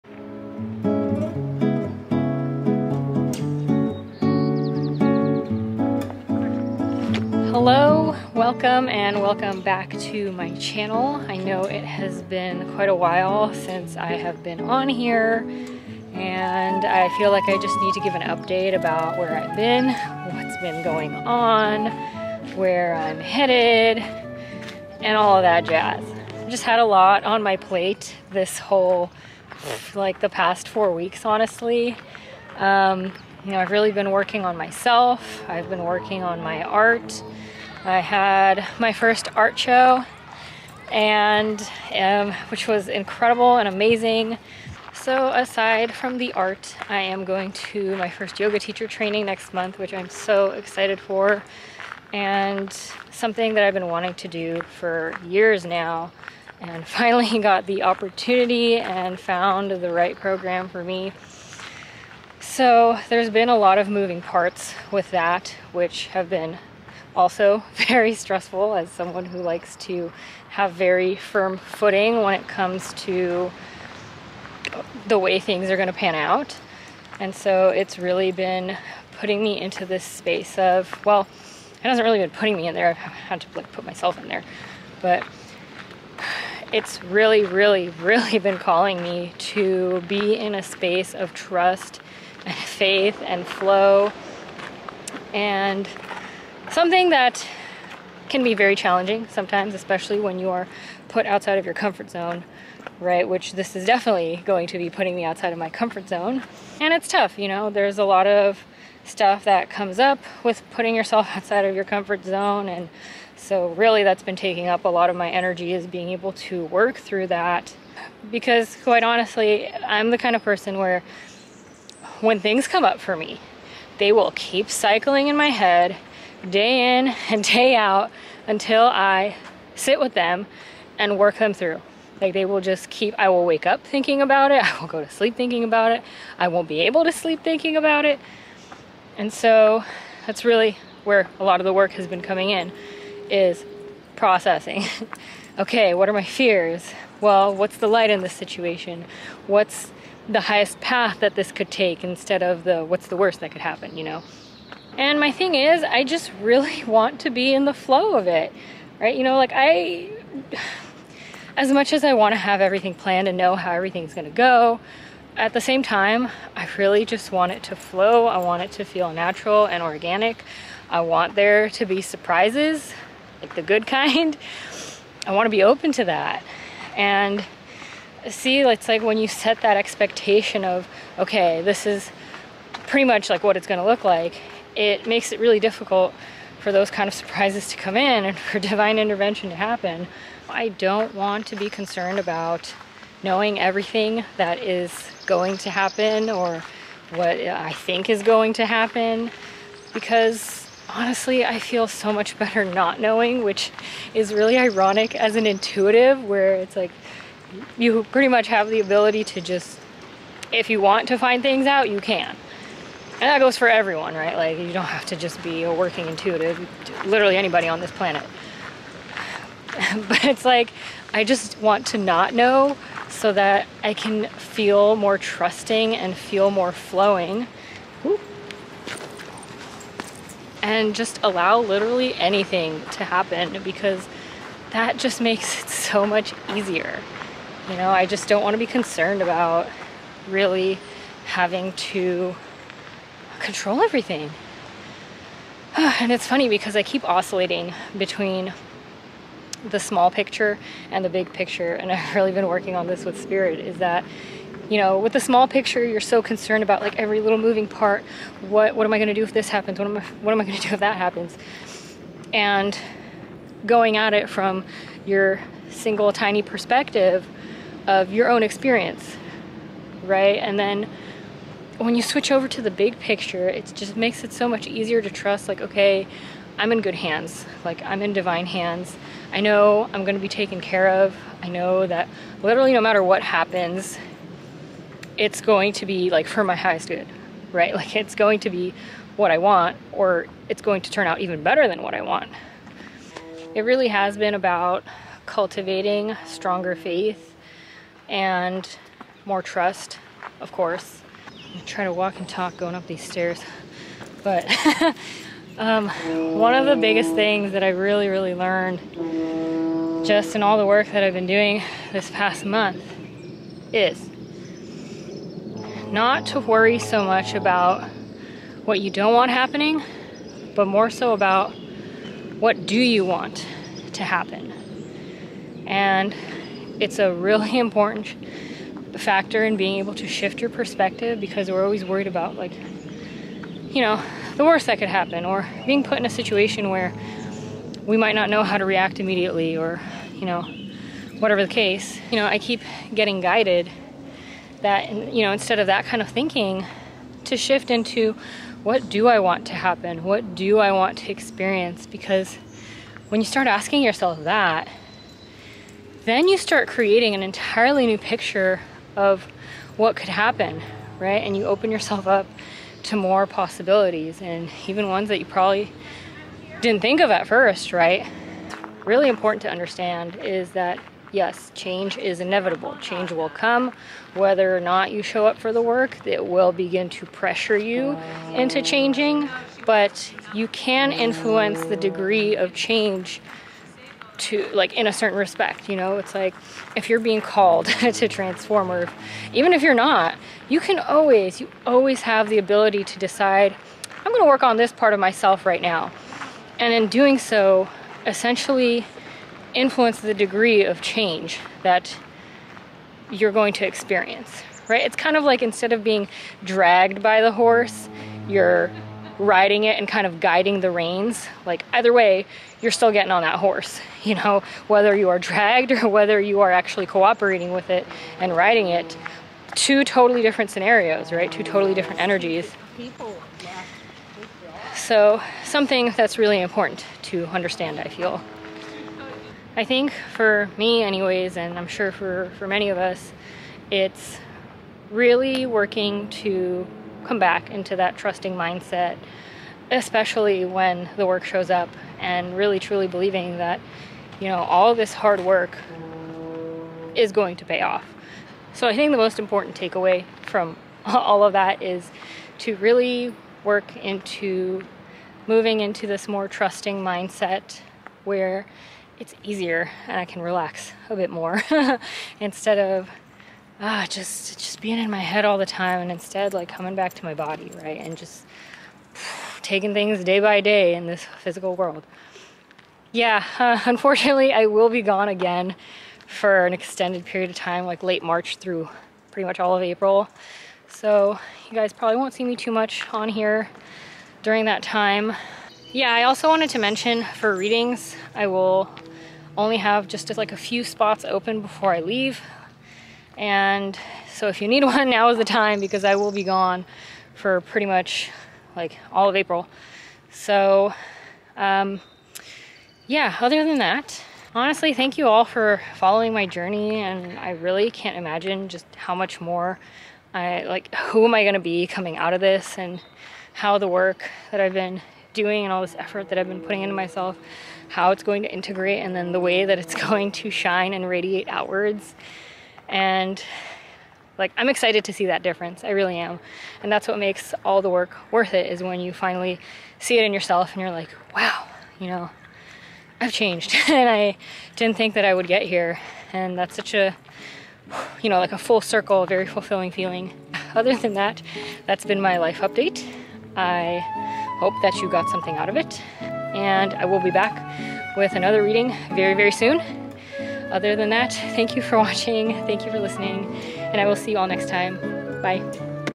Working on TRUSTING the process | a spiritual journey
Hello all,
I know I've gone quiet for a bit, this is why.

Limited spaces for readings this month! Aside from that, I hope Pisces season is treating you as well as can be 🐠

Love,
Kristina